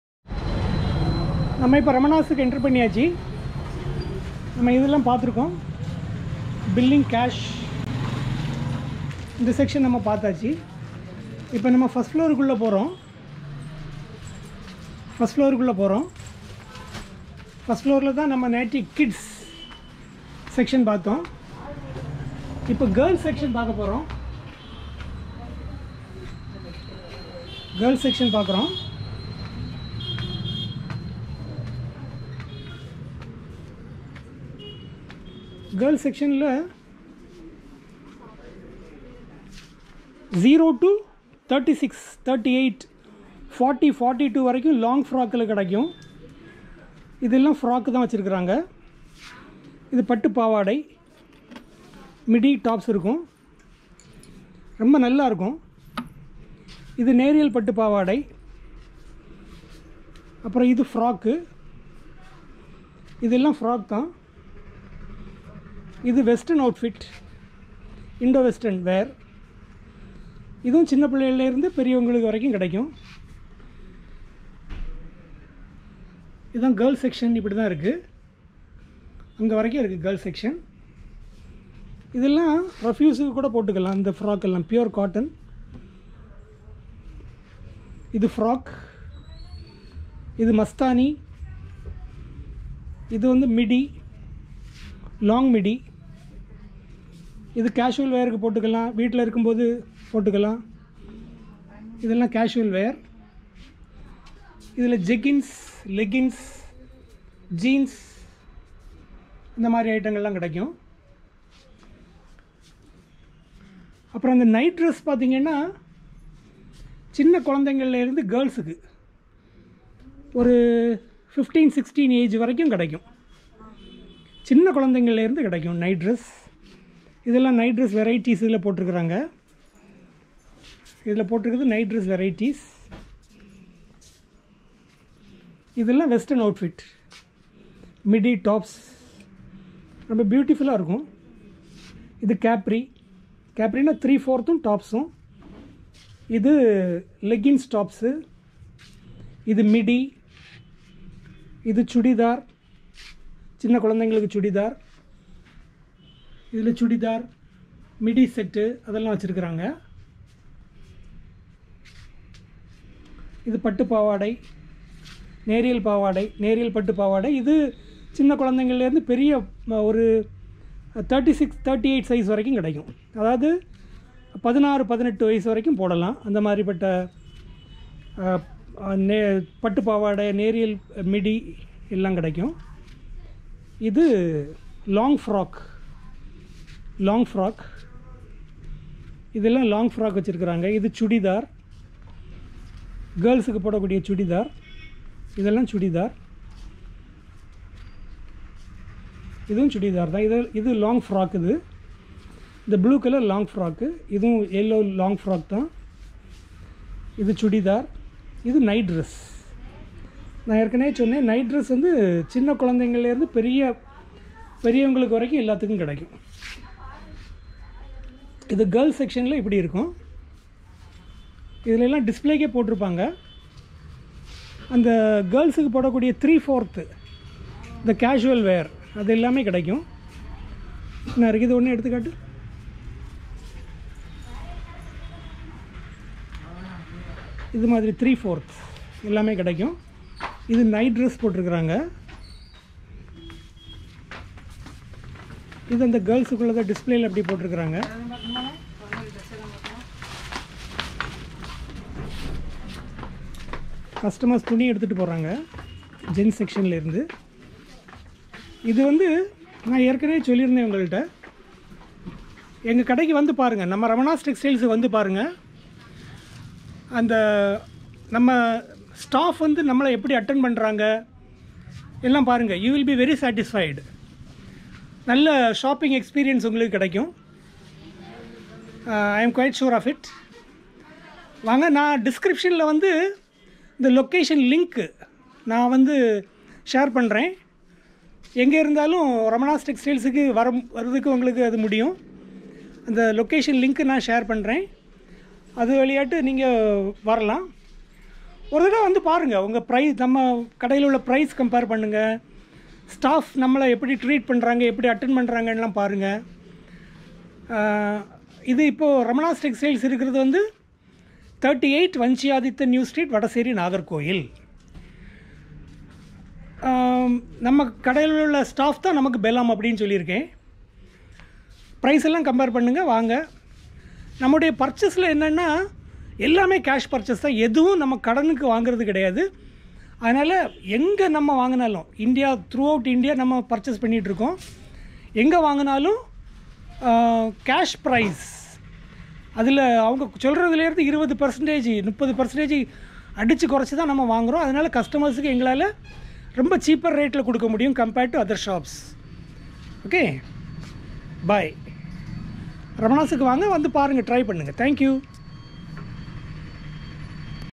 GoPro 8 shoot. Ramanas. Girls' section.
Review of GoPro Hero 8 Black.
Cinematic mode.
Super wide angle.
Ramanas Girls'section.
38, Vanchi Athithan New Street,
Vadasery, Nagercoil.